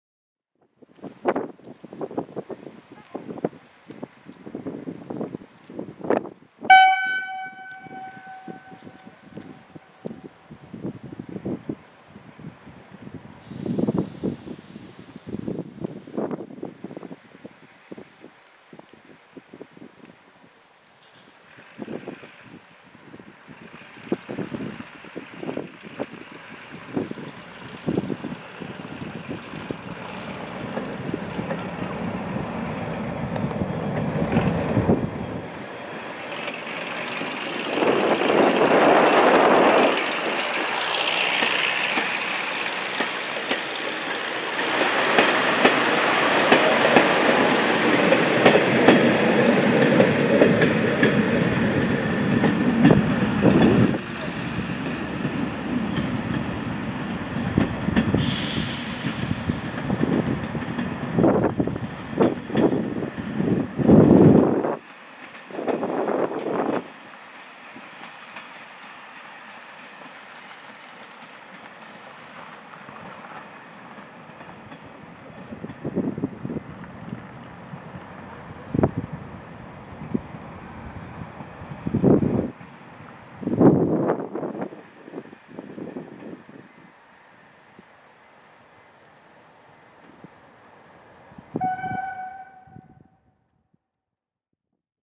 2021-11-21 - Vlp - 1923 - Dm7 4209+4185+4215
HMVY Ry:n operoima MUS 1923 lähtee Vilppulasta kohti Mänttää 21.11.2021. Junan kokoonpanona oli Dm7 4209+4185+4215.